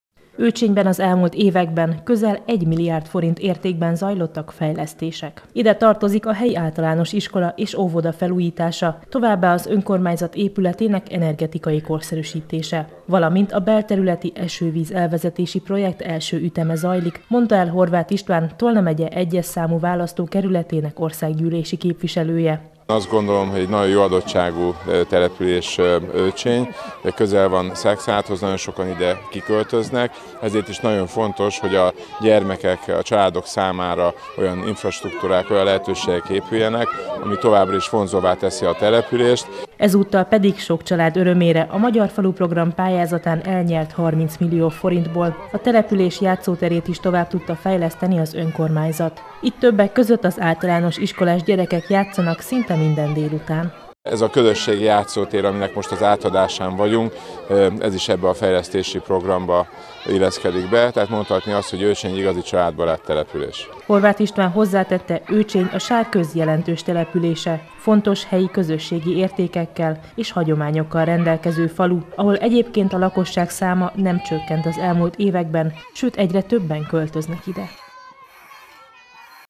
Új játszótér Őcsényben
Új játszóteret kaptak az őcsényi gyerekek. A település ugyanis 30 millió forintot nyert el a Magyar falu program pályázatán. Horváth István Tolna megye 1-es számú választókerületének országgyűlési képviselője azt mondja, a fejlesztésnek köszönhetően Őcsény igazi családbarát település lett.